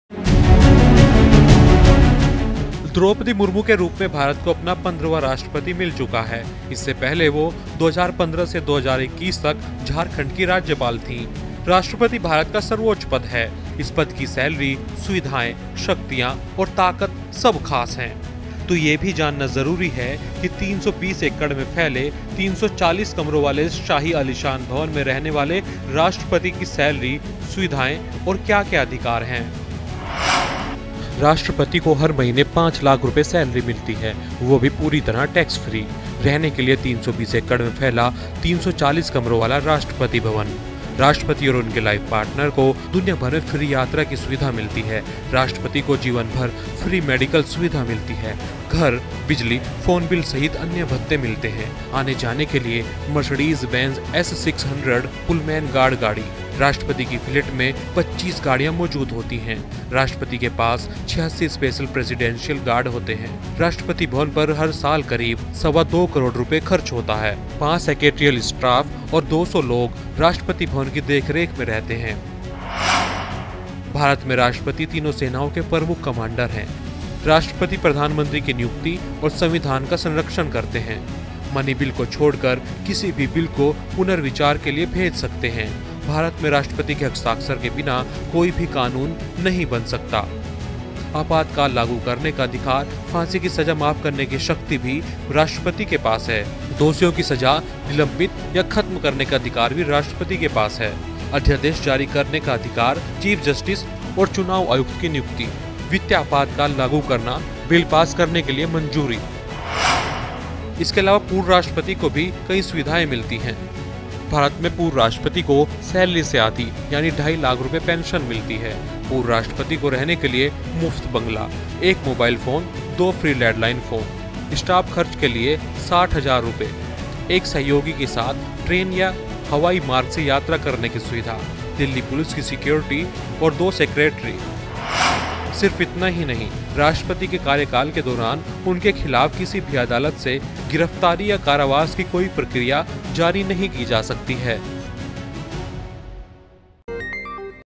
President of India: 5 लाख सैलरी, 340 कमरों का भवन, जानें राष्ट्रपति और पूर्व राष्ट्रपति की सुविधाएं
President of India: क्या आपको पता है कि भारत के प्रथम नागरिक को कितनी सैलरी (President of India Salary) मिलती है ? उनके अधिकार क्या-क्या हैं ? राष्ट्रपति और पूर्व राष्ट्रपति को सुविधाएं कितनी मिलती है ? इस रिपोर्ट में जानें हर जवाब हैं.